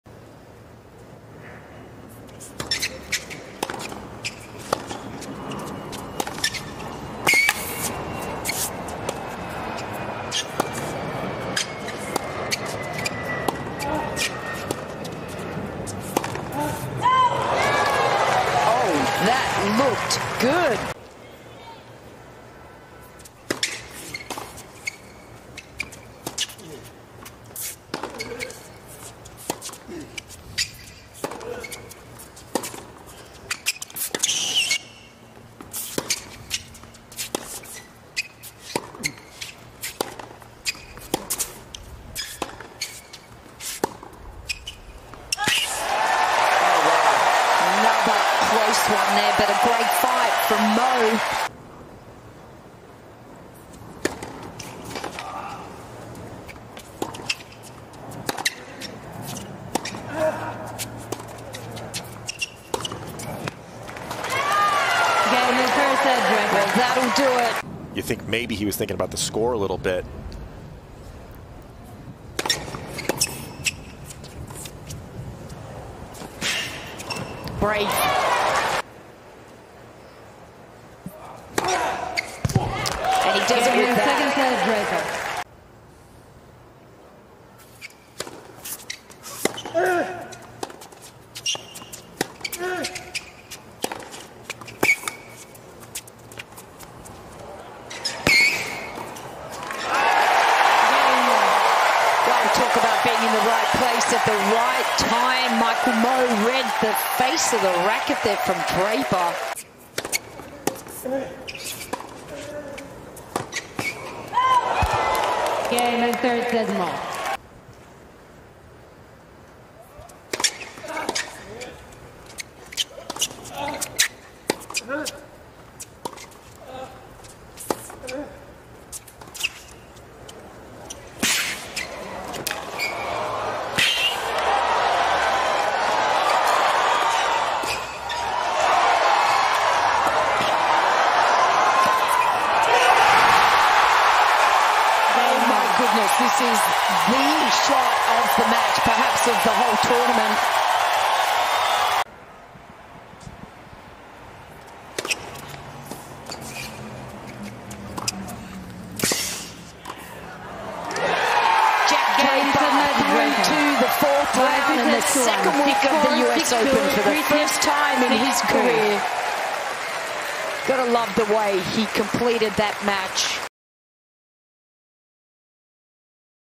Michael Mmoh vs. Jack Draper Highlights | 2023 US Open Round 3
Watch the highlights of Michael Mmoh vs. Jack Draper in Round 3 of the 2023 US Open. These highlights are presented by Cadillac.